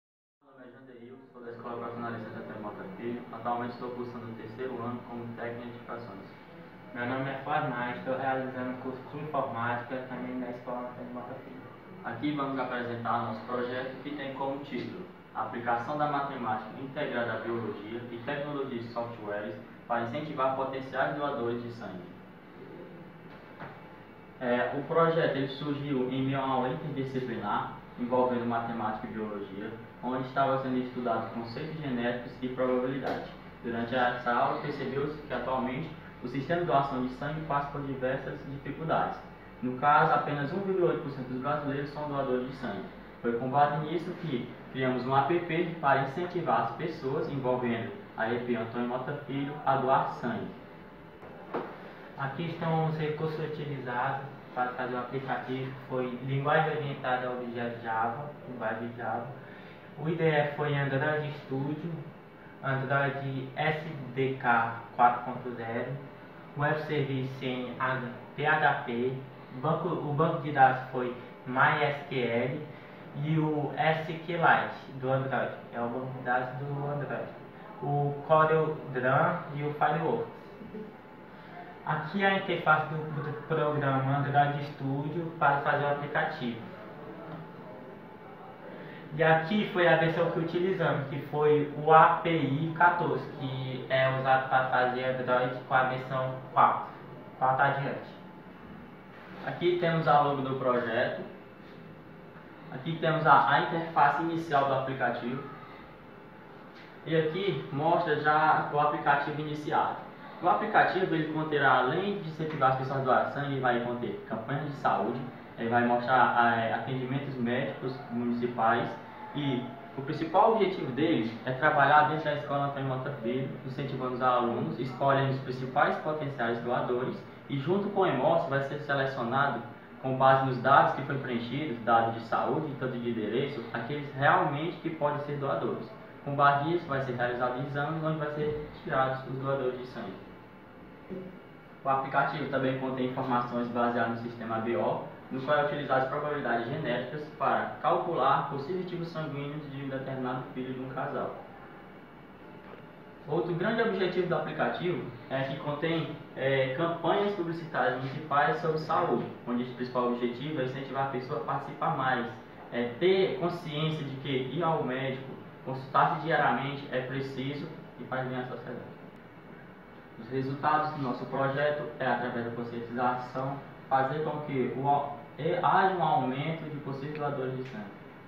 Projeto integrado de Aplicação de Software com Matemática e Saúde
Projeto escolar da Escola Estadual de Educação Profissional Antonio Mota Filho.

APLICAÇÃO DA MATEMÁTICA INTEGRADA À BIOLOGIA E TECNOLOGIA DE SOFTWARES PARA INCENTIVAR POTENCIAIS DOADORES DE SANGUE